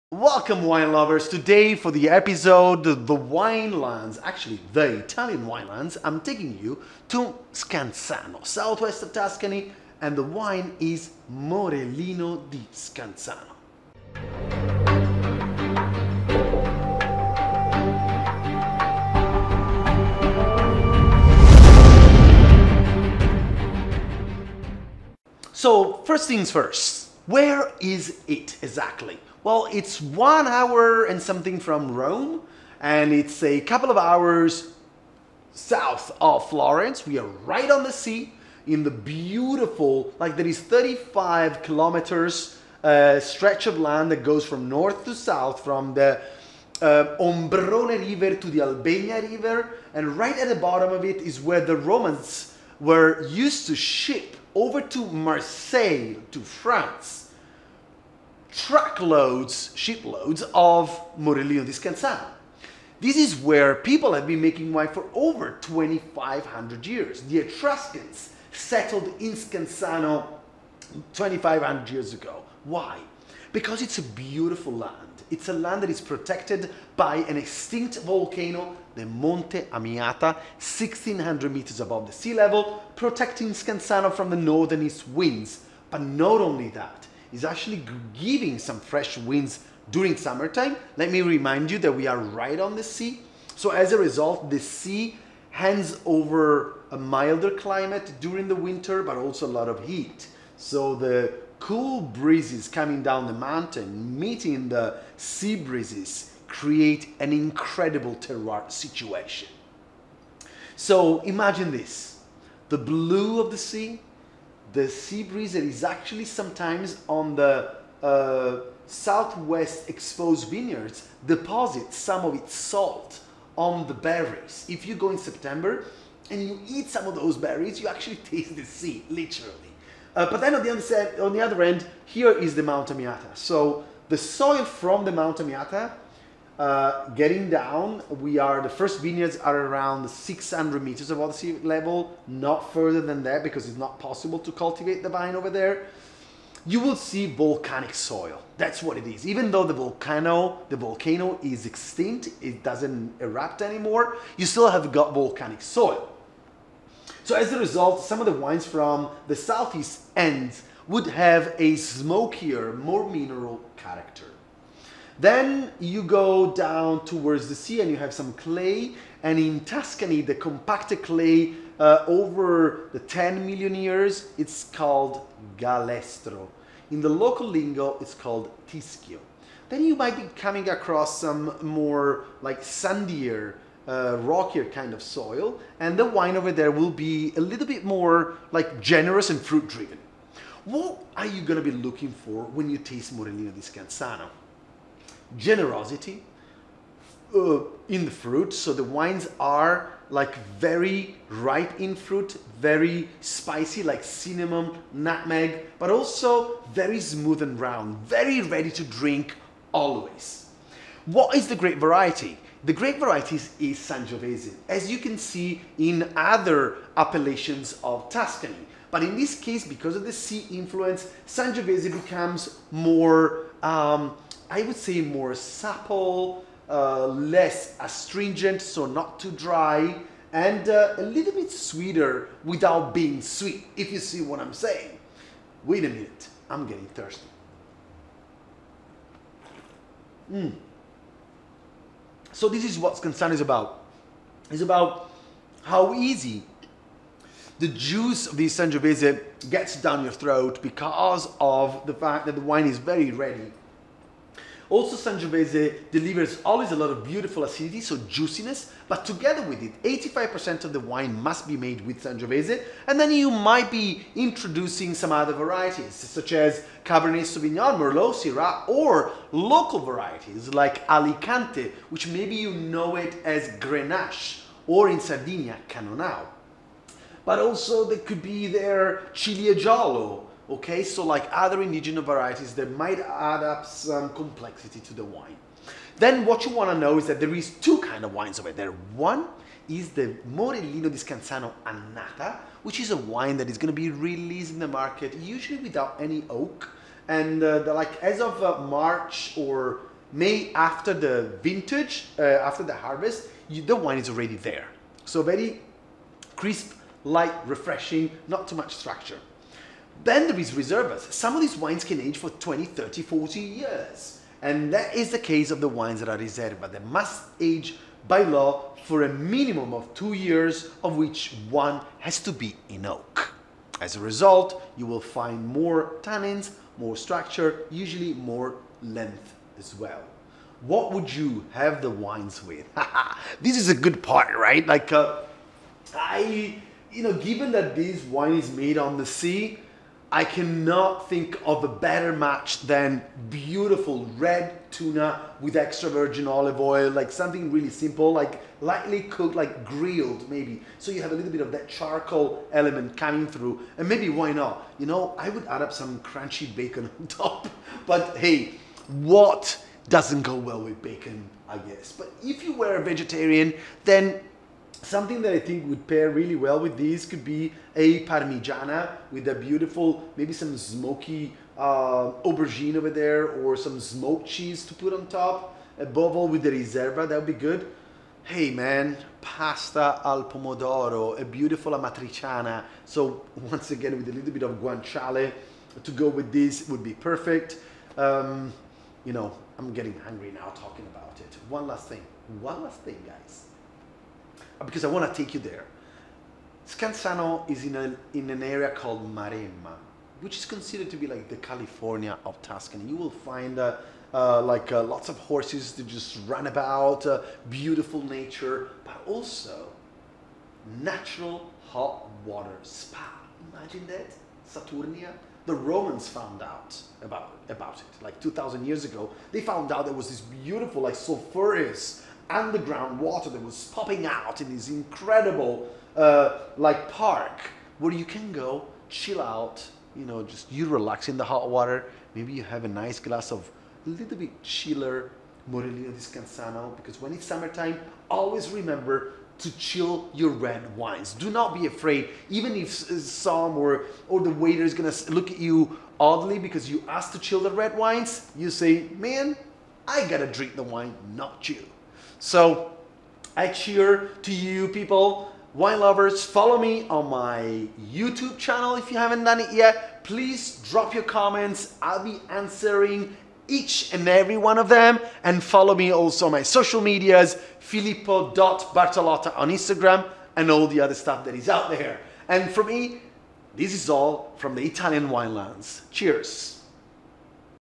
Wine Tasting - Italian Wine Lands - Discovering Italian Wines - Ep.1 Morellino di Scansano
Not an usual wine tasting. Wanna find out what wine of Italy is about? You are in the right place. With Filippo Bartolotta you will be lead to the most famous italy wine lands.

In this episode, Filippo will run you to the south of Tuscany in the Maremma, with the Morellino di Scansano Appellation.

▶ LIKE ON FB: Filippo Bartolotta